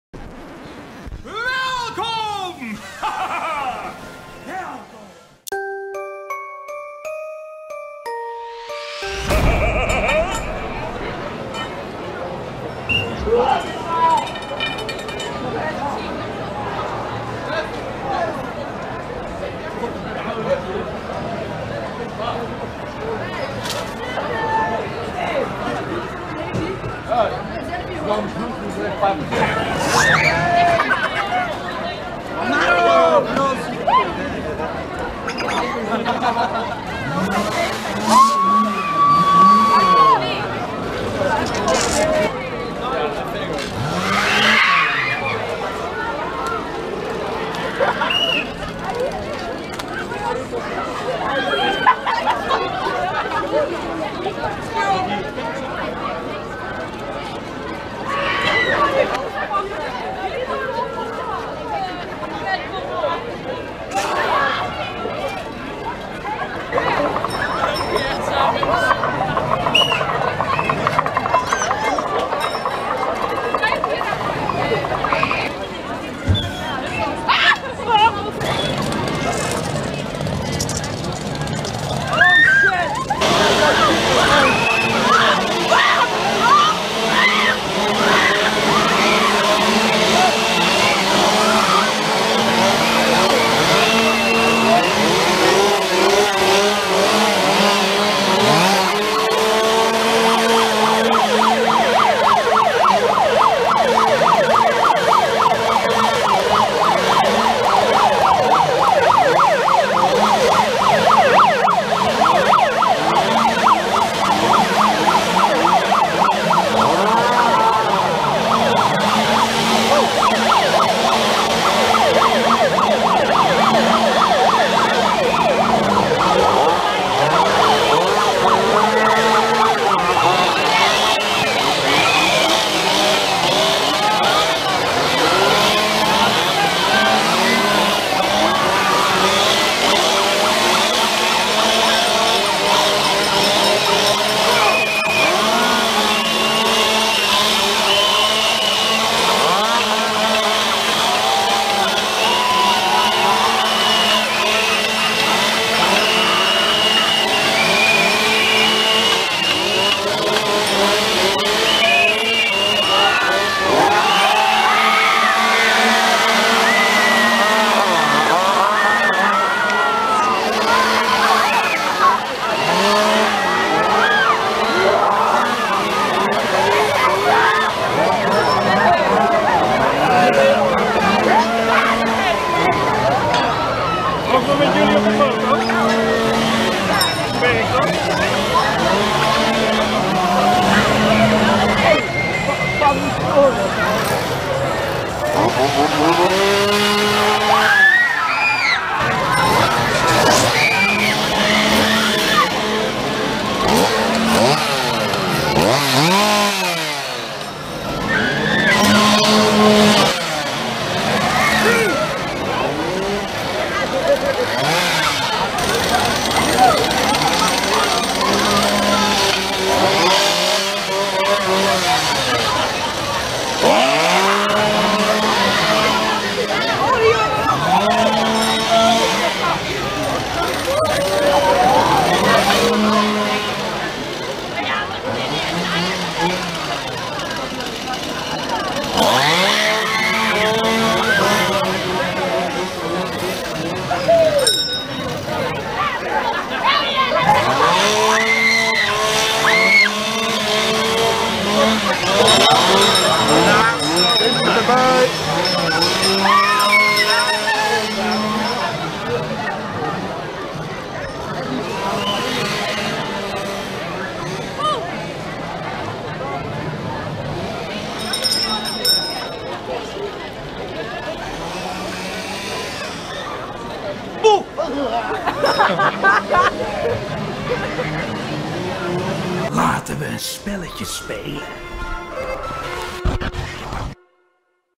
Halloween Fright Nights 2018 - De uitgang na 23 uur (Donderdag 25-10-2018)
Van 6 t/m 28 oktober 2018, elke vrijdag, zaterdag en zondag, en donderdag 25 oktober, speel jij met je leven in Walibi Holland. Bezoek tussen 18:00 uur tot 23:00 uur de zes gruwelijke Spookhuizen, acht angstaanjagende Scare Zones en Eddie’s Roast Show in zijn eigen area!

Eddie draait aan zijn Wheel of Fear, en het lot bepaalt. Durf jij de gok te wagen?